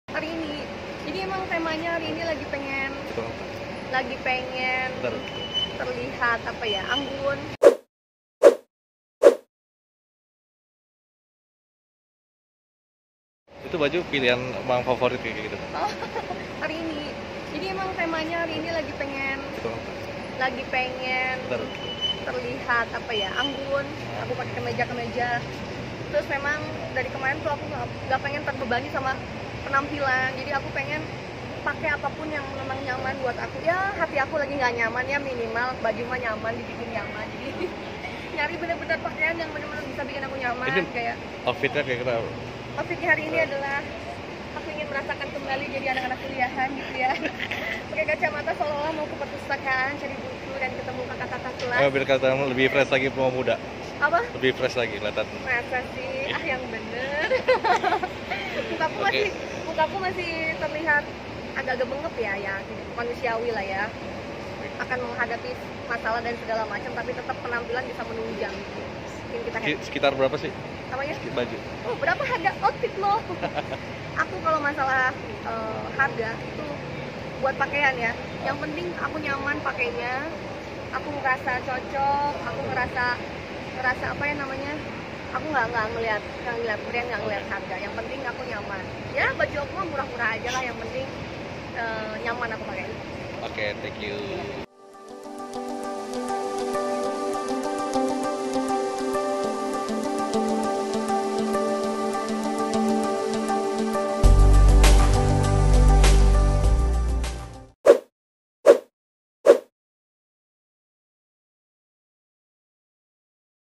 Celeb Review: Jenita Janet Kangen Busana Kuliahan, Ini Alasannya
Tampil apik dan menarik adalah tuntutan bagi seorang figur publik seperti Jenita Janet. Kali ini ia memilih model busana anak kuliahan yang simpel dan menarik. Diluar semuanya kriteria yang paling menentukan dalam memilih busana adalah soal kenyamanan. Yuk lihat OOTD alias outfit of the day Jenita kali ini. Siapa tahu bisa jadi inspirasi buat kamu.